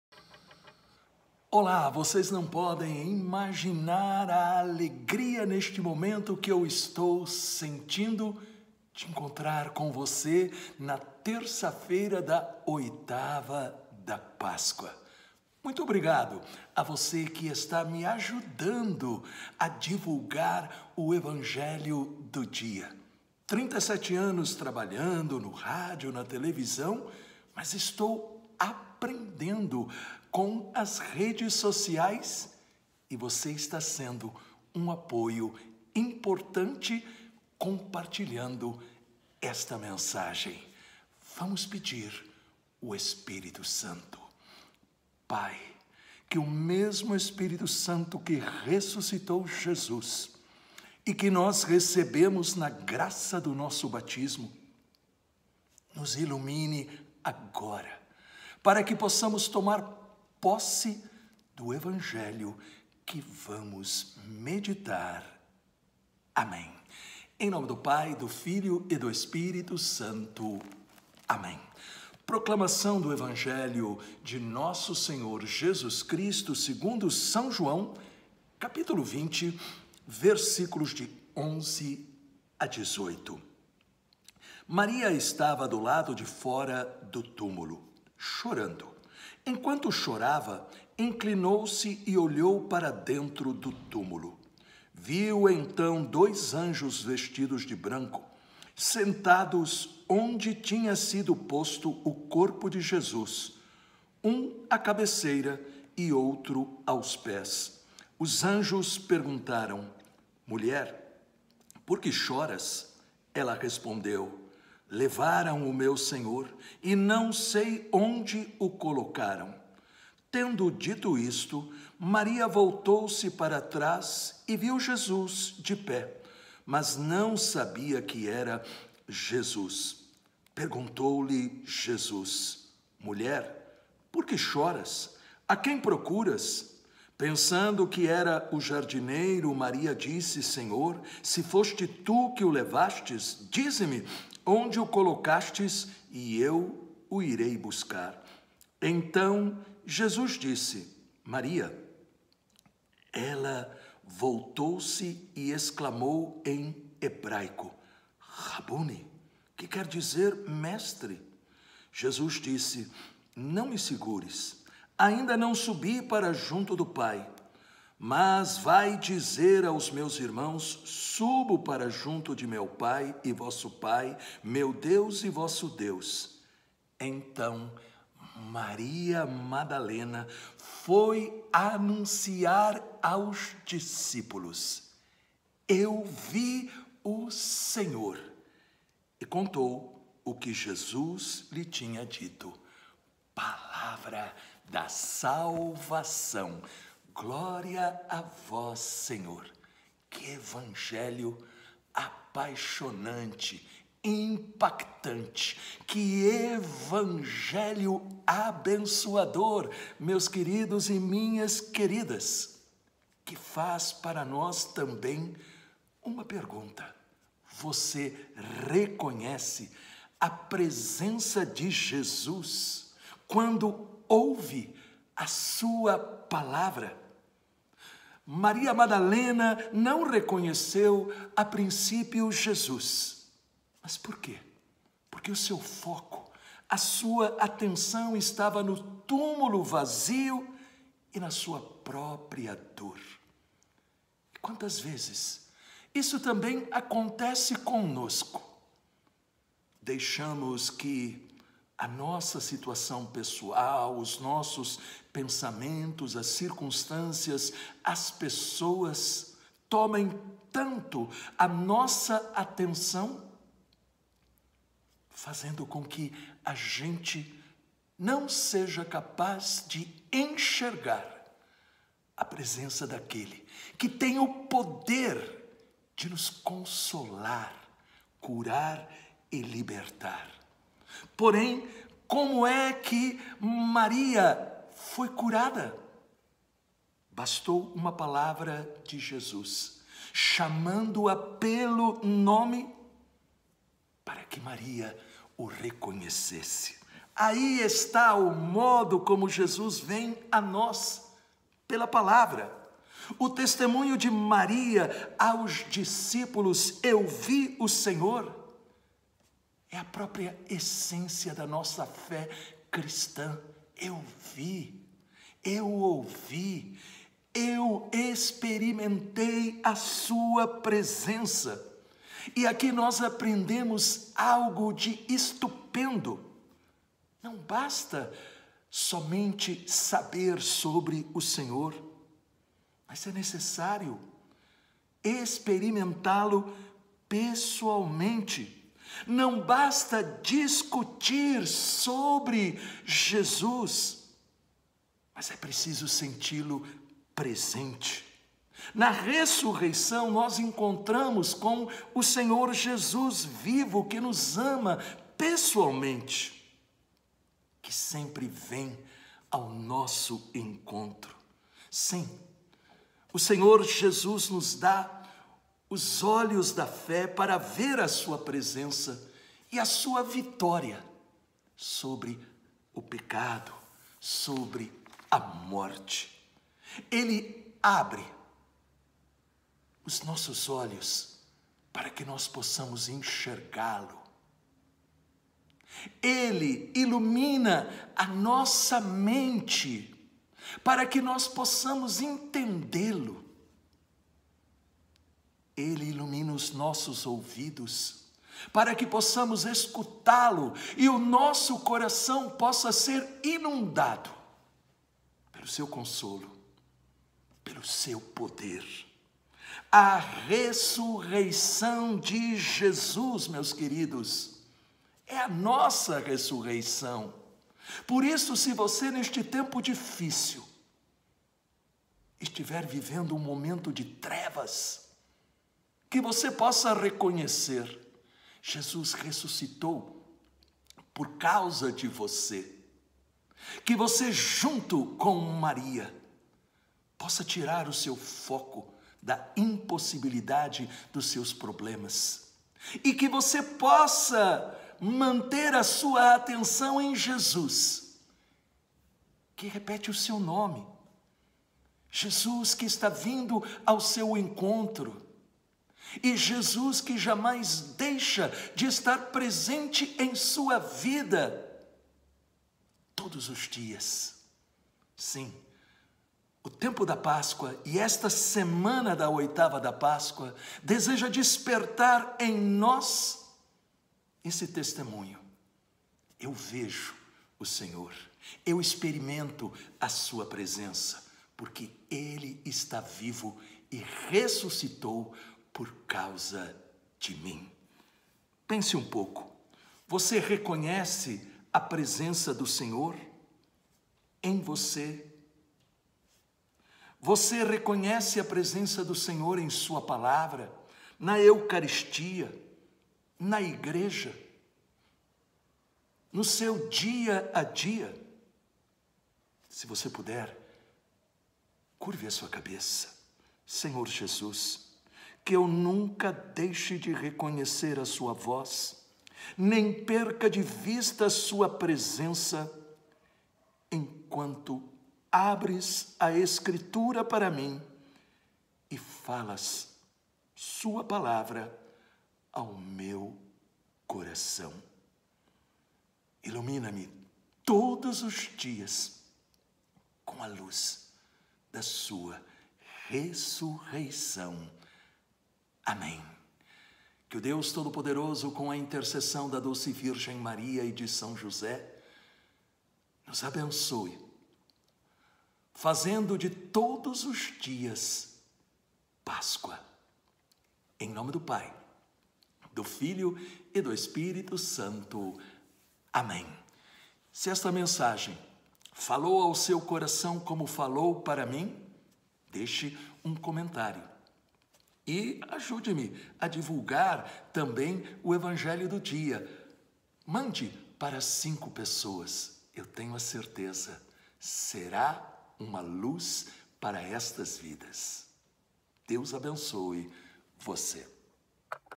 EVANGELHO DO DIA 19/04 (3ªfeira da 8ª da Páscoa): VER JESUS HOJE! Jo 20,11-18
A fé nos faz enxergar e experimentar a presença de Jesus ressuscitado. Porém, como Maria Madalena é preciso testemunhar que Ele está vivo. Deixe um comentário e compartilhe esta mensagem.